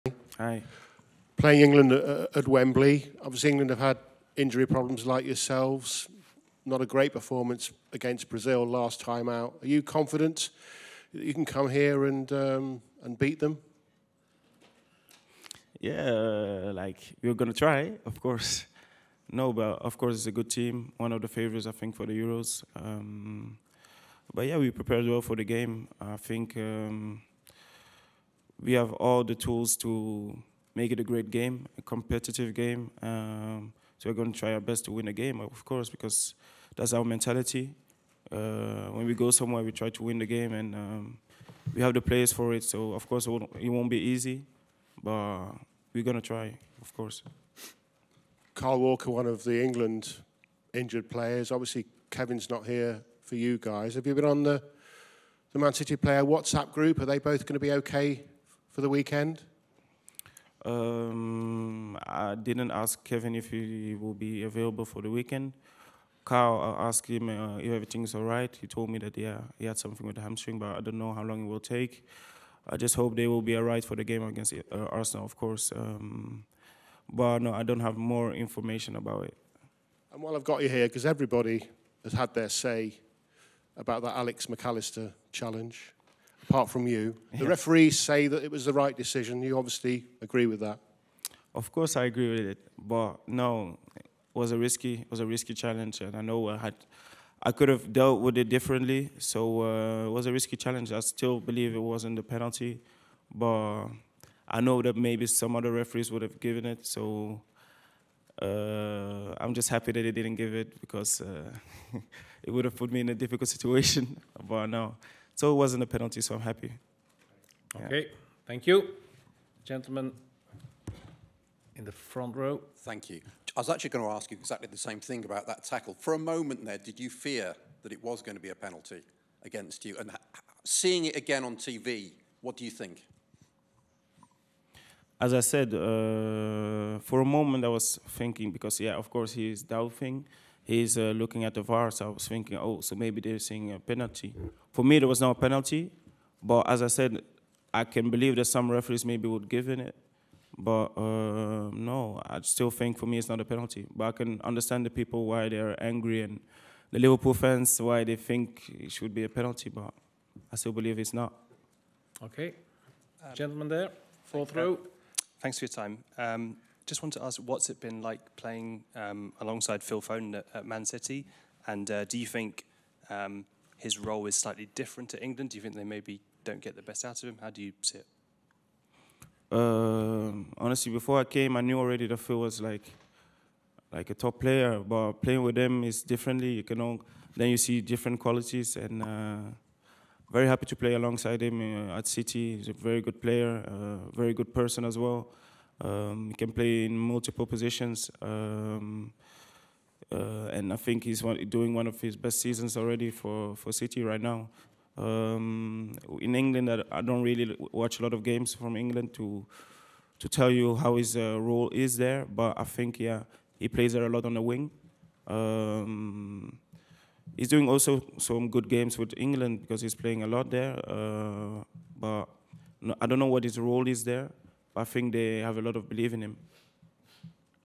De Bruyne and Walker FIT for Arsenal? | Man City winger Jeremy Doku ahead of England v Belgium
Man City winger Jeremy Doku speaks ahead of England v Belgium and says he is hopeful that City duo Kevin De Bruyne and Kyle Walker will be fit to face Arsenal on Sunday.

TIMESTAMPS
0:00- Intro

0:17- Confident that you can beat England? Yeah we are going to try! A good team for the euros but we prepared well and have all the tools to make it a great game and try our best to win it

1:00- Walker and De Bruyne okay for the weekend? I didn’t ask Kevin and Walker said he had something in his hamstring, hope they will be alright for Arsenal

1:29- Alexis Mac Allister challenge? It was a risky challenge and I know I could have dealt with it differently but still believe it wasn’t a penalty

2:14- Did you worry it would be a penalty? For a moment I was thinking maybe they’re seeing a penalty but for me it’s not a penalty, maybe some referees would give it

2:59- Foden’s role for Man City and England? Before I came I knew he was a top player, very happy to play alongside him, one of his best seasons for city, don’t watch a lot of England but he plays a lot on the wing, good games with England too